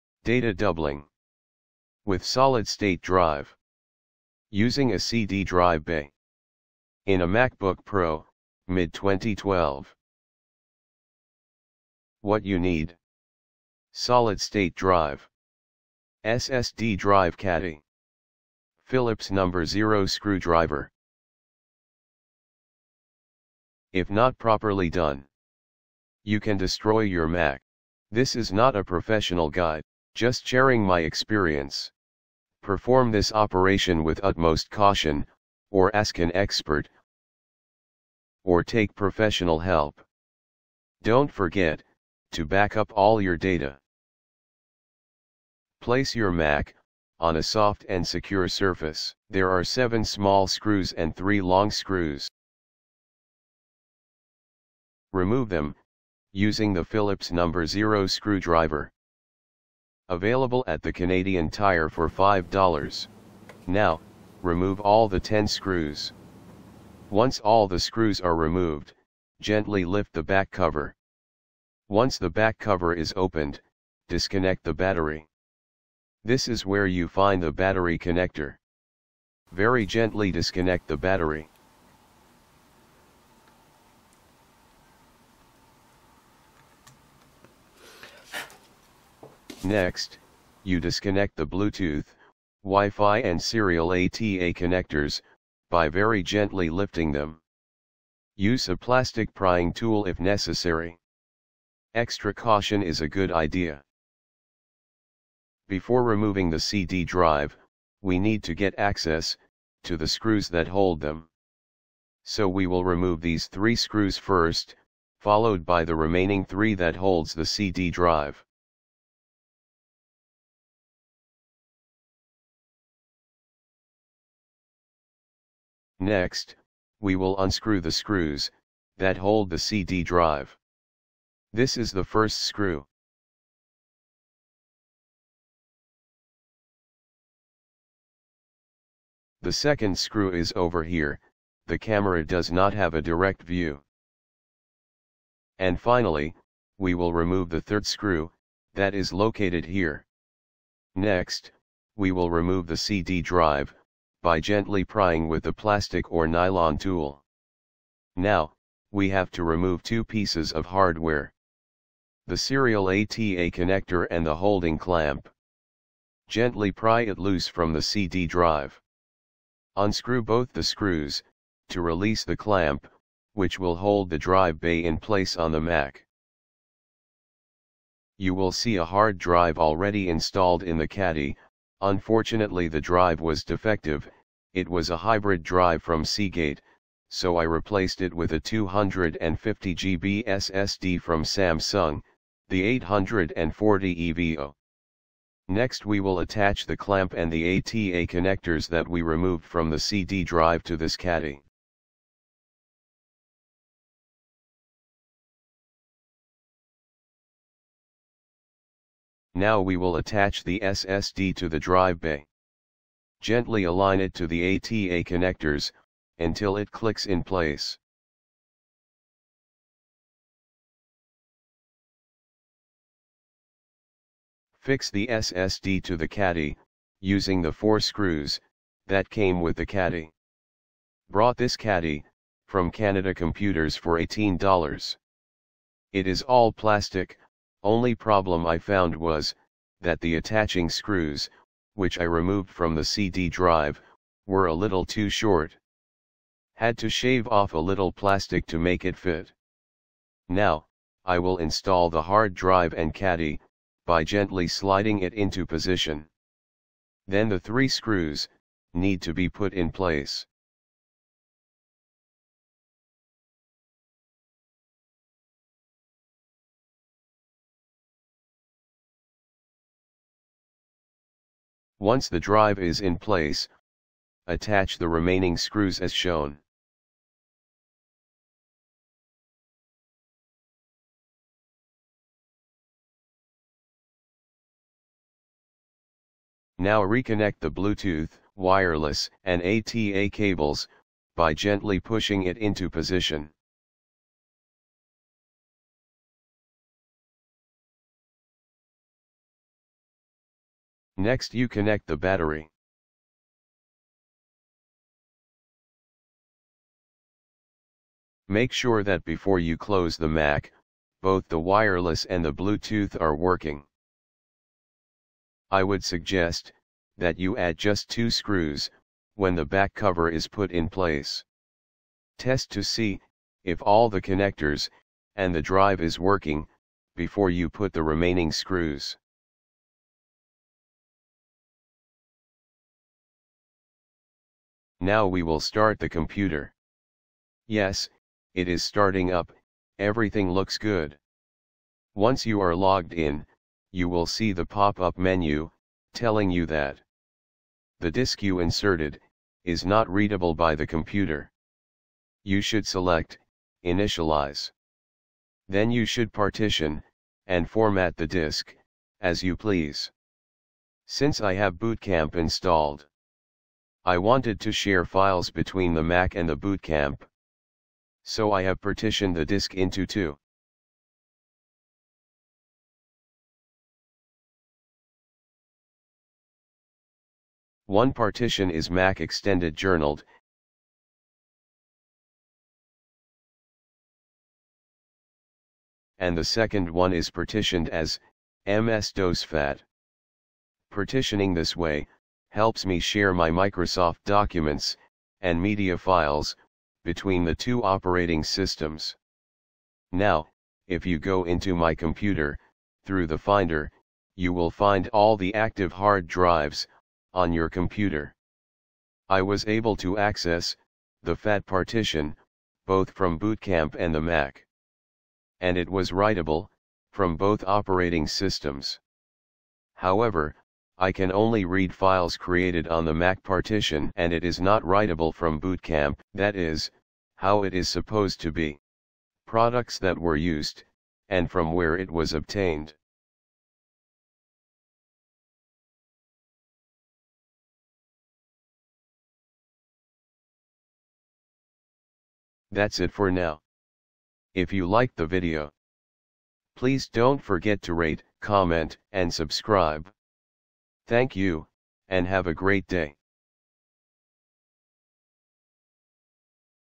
Macbook Pro | Add a Second Drive | Replace your Slim CD Drive With SSD
This video shows you how I did some data doubling for my Mac Book Pro with an SSD using Caddy in the CD drive.
The video shows how it was done.
Always seek professional advice before proceeding to double your data.
This video is not meant to be a professional how to video.
For those that are curious, this is a great opportunity to see what happens and how it happens.
This is another way to share documents between windows and mac.